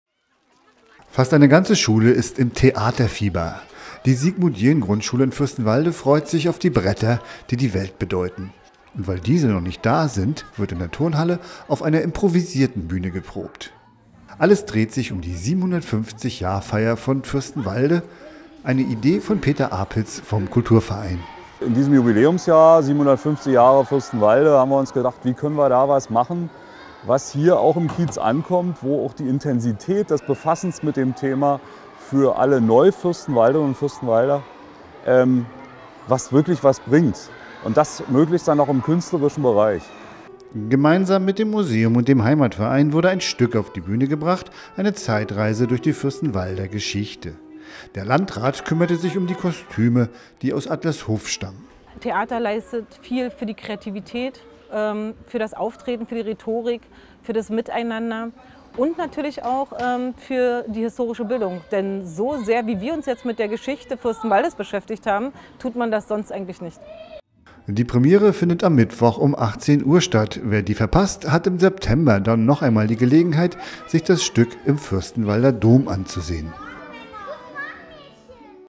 Schüler fliegen durch die Zeit
Ein Highlight im 750. Jubiläumsjahr von Fürstenwalde gibt es schon in dieser Woche. Die Schüler der Sigmund-Jähn-Grundschule bereiten sich gerade darauf vor. Die Aufregung ist groß, den es wird eine Zeitreise durch die Geschichte der Domstadt.

@Fürstenwalder Kulturverein e.V.